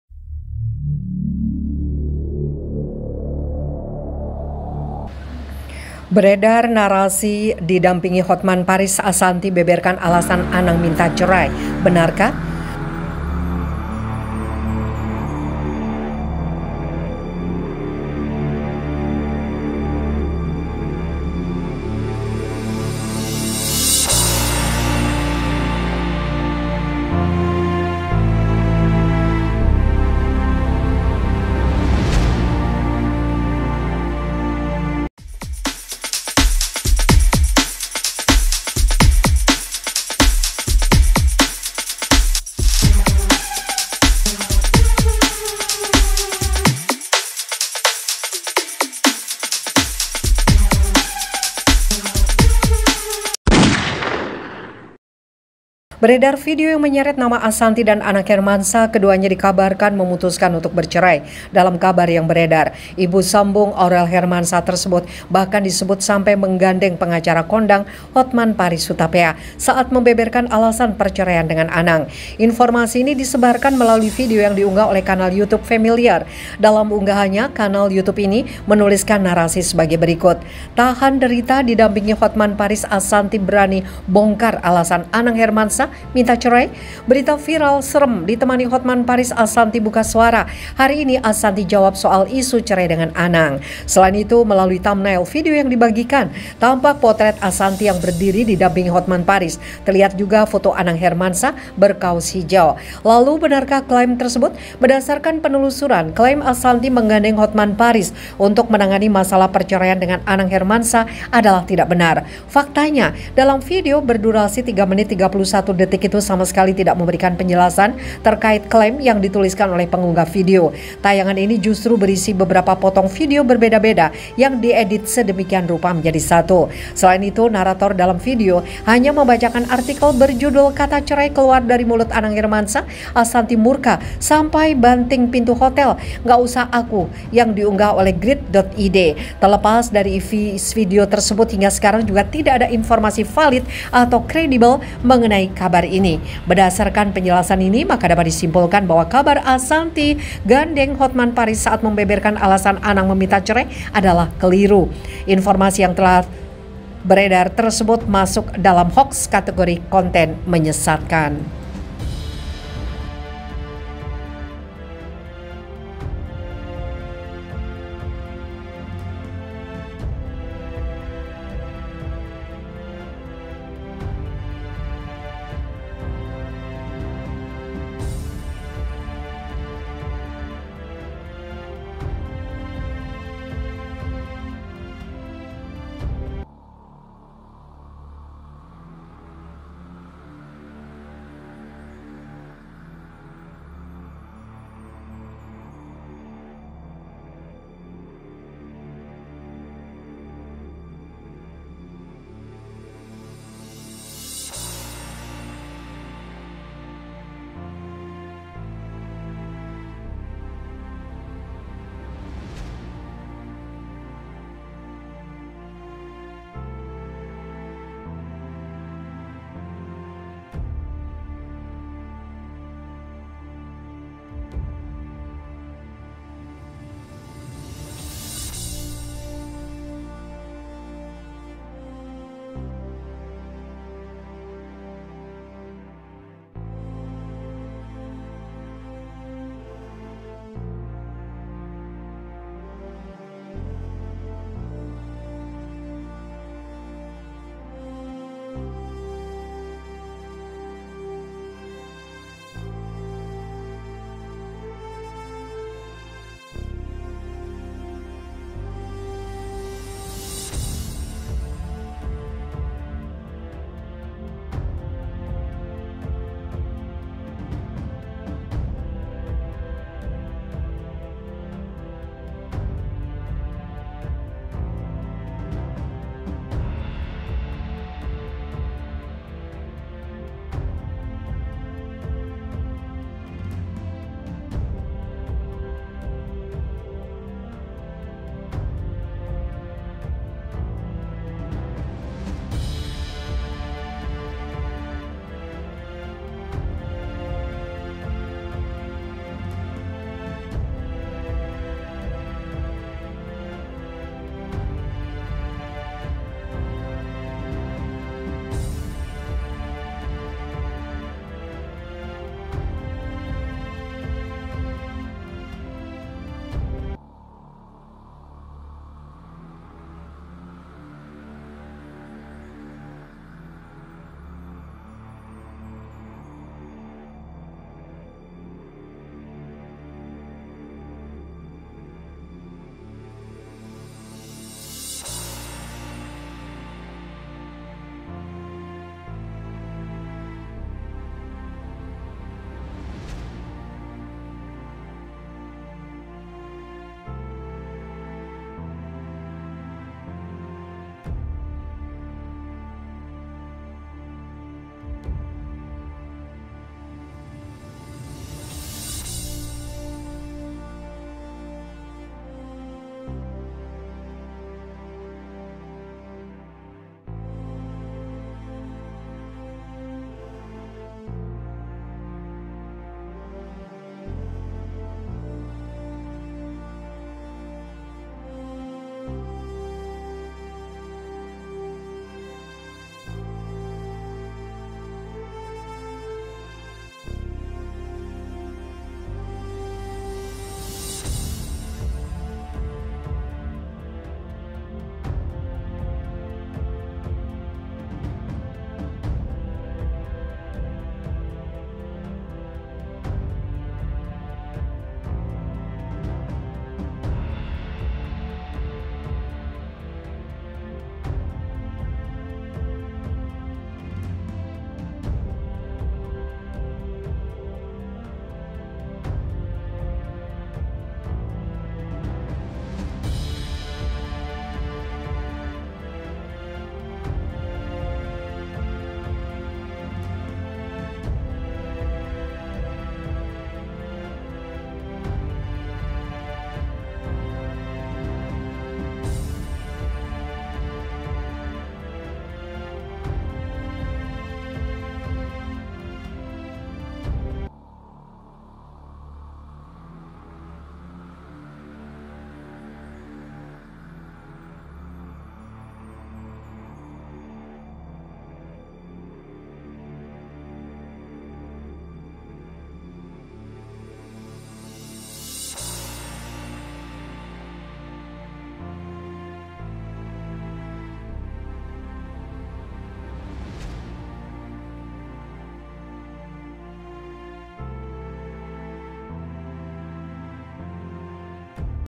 ASHANTY AKHIRNYA BERANI,DI DAMPINGI HOTMAN PARIS ASHANTY BONGKAR ALASAN ANANG MINTA CERAl
ASHANTY AKHIRNYA BERANI,DI DAMPINGI HOTMAN PARIS ASHANTY BONGKAR ALASAN ANANG MINTA CERAl-GOSIP ARTIS HARI INI